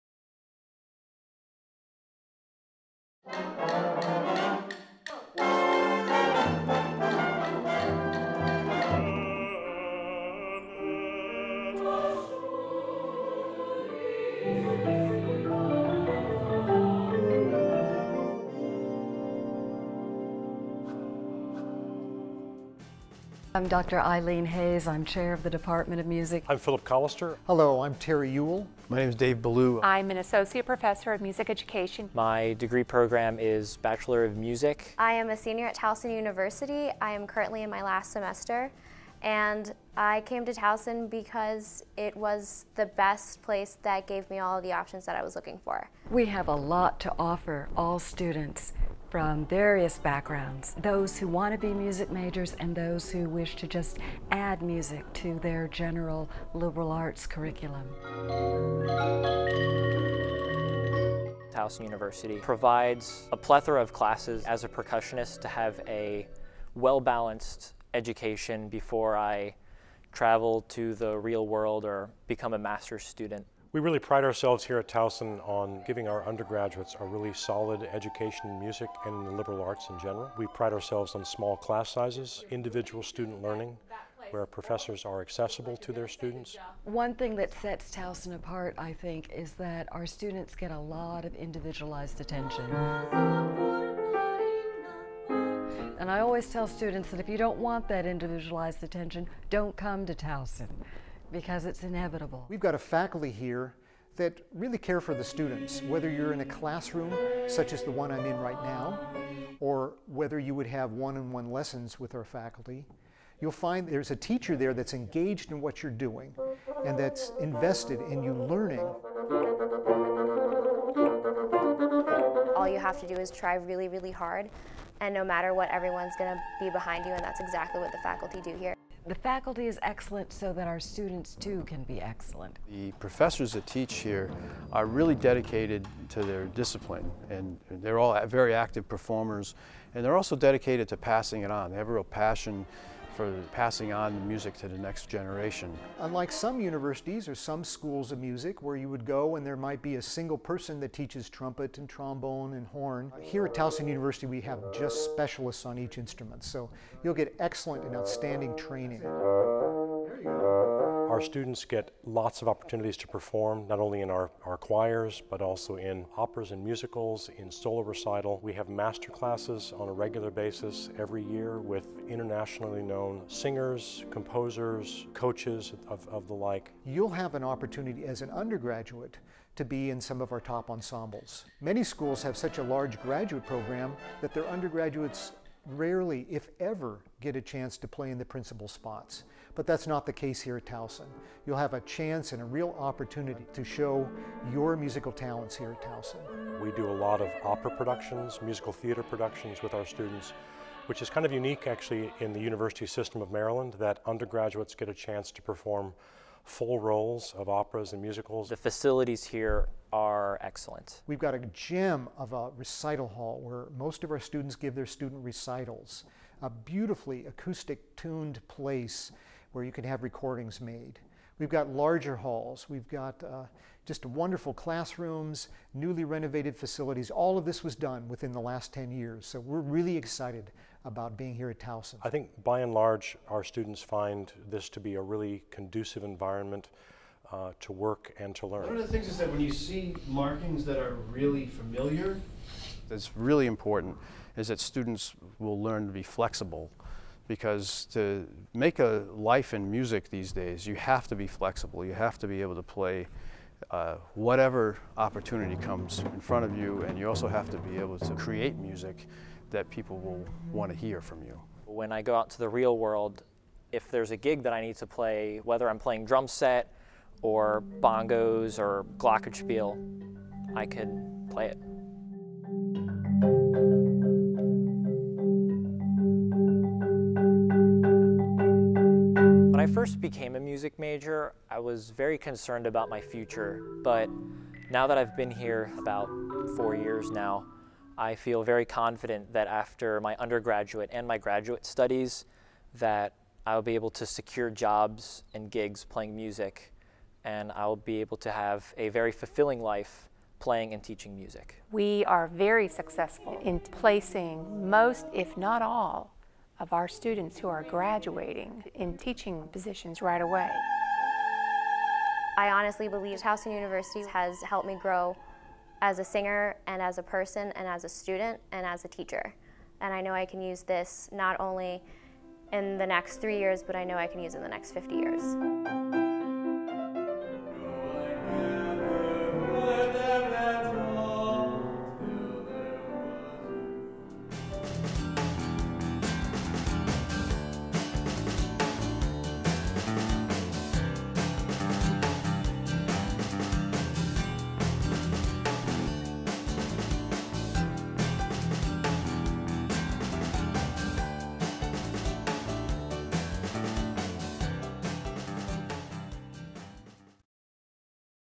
Department of Music: Towson University
For more information about Department of Music at Towson University go

Produced by Towson University Electronic Media and Film students Chris Brown, Josh Franz, and Nick Jameson in Community Video Service Learning course EMF 437 with Dr. Dave Reiss.